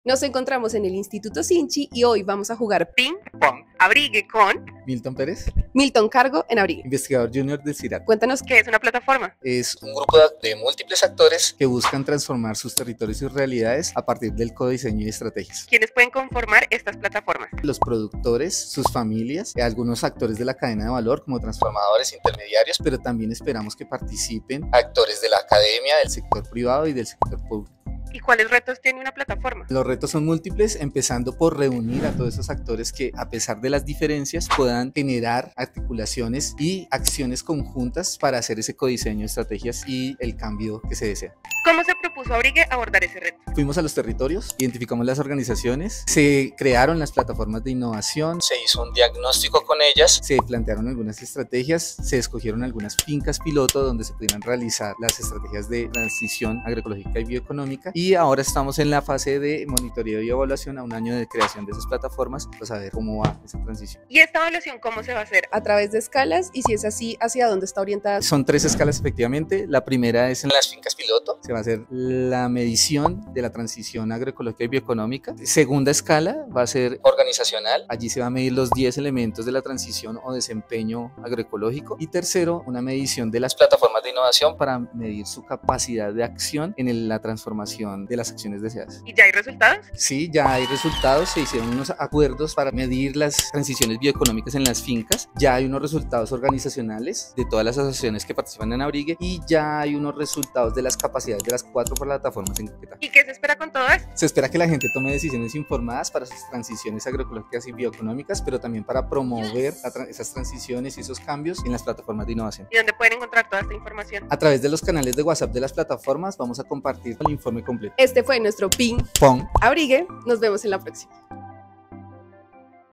Validación de Indicadores - Plataformas de Innovación Caquetá Proyecto ABRIGUE
¡Tenemos devolución de resultados! 👏🏽 El pasado mes de noviembre nos reunimos con los actores de las PlataformasDeInnovación de Caquetá📍, para construir los indicadores de evaluación que les orientarán en la toma de mejores decisiones para las TransicionesAEBE🟢 .

Hoy les contamos cuál ha sido este camino recorrido y les facilitamos el informe donde encontrarán la ampliación de las metodologías, los indicadores y los resultados.📊

ABRIGUE es financiado por la @UEenColombia 🇪🇺, y es coordinado por el InstitutoSINCHI, en articulación con @agrosaviatv , @MincienciasCanalOficial , @LeCIRAD y la Universidad Tecnológica del Chocó